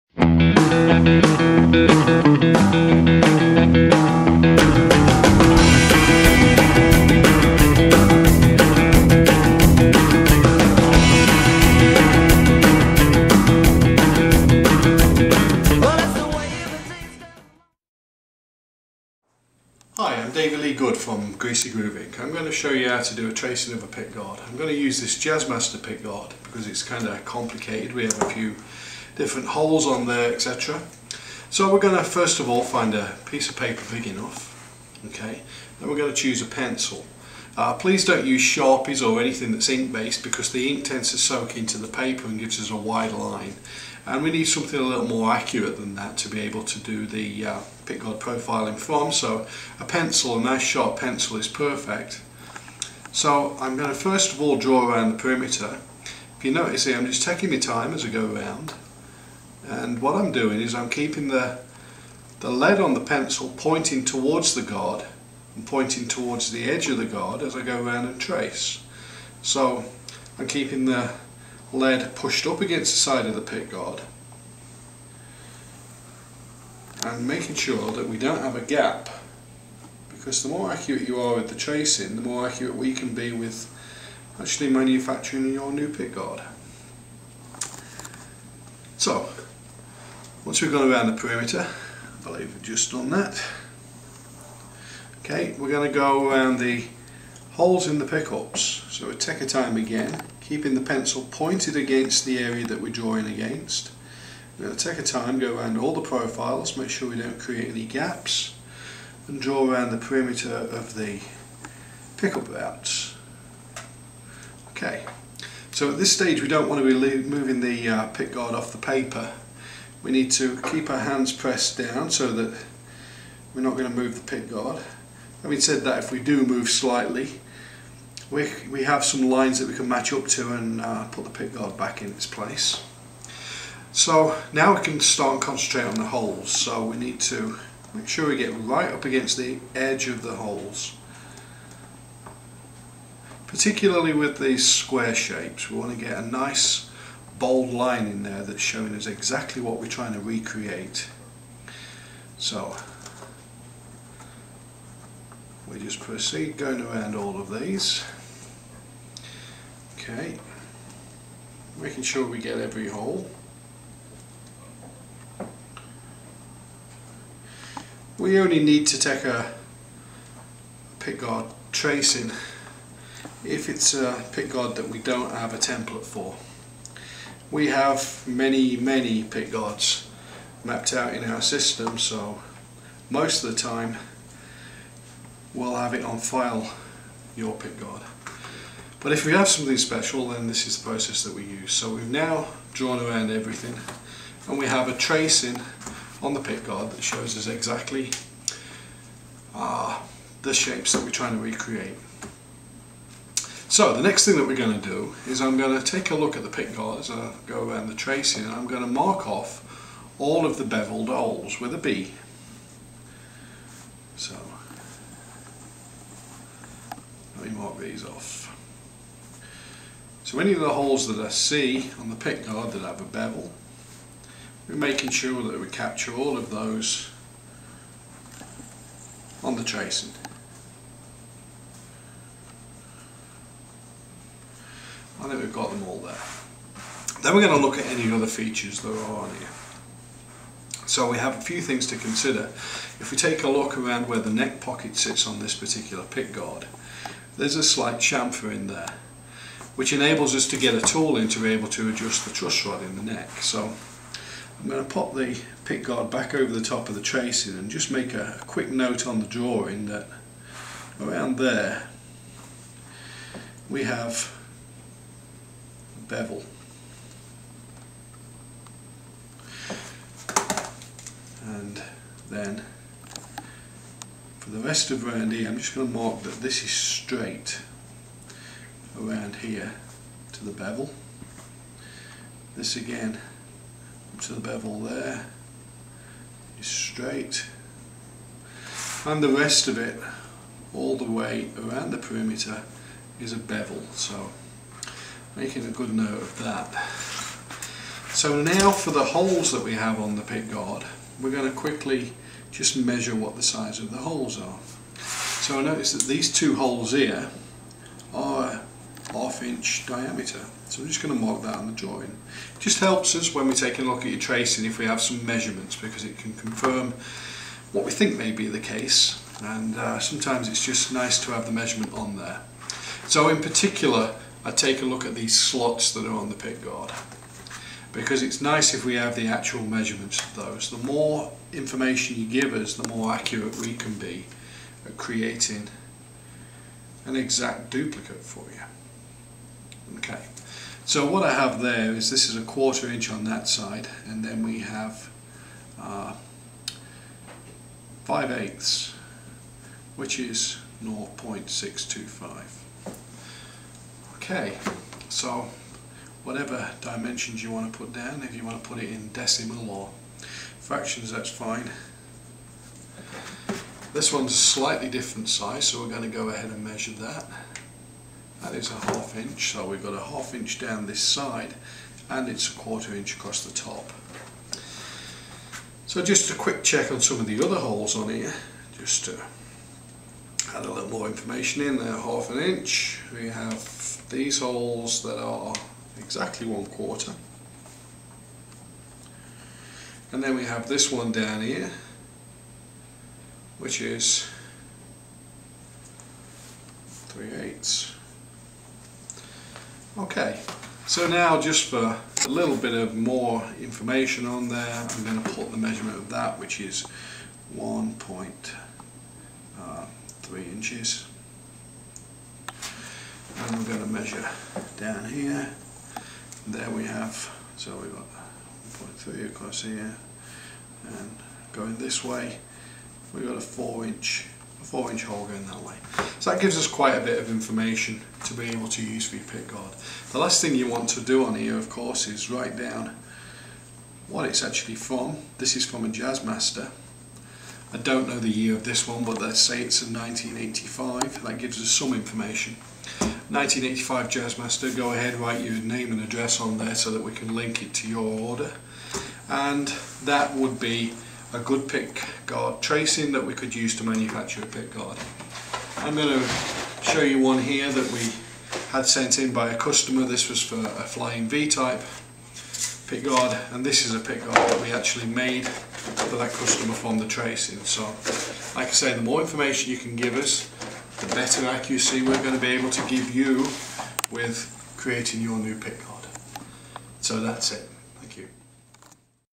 How to Trace a Pickguard or Cover
Instructions on how to trace a pickguard or other guitar part so you can send it to Greasy Groove to have a brand shiney new one made!    Davey lee Goode.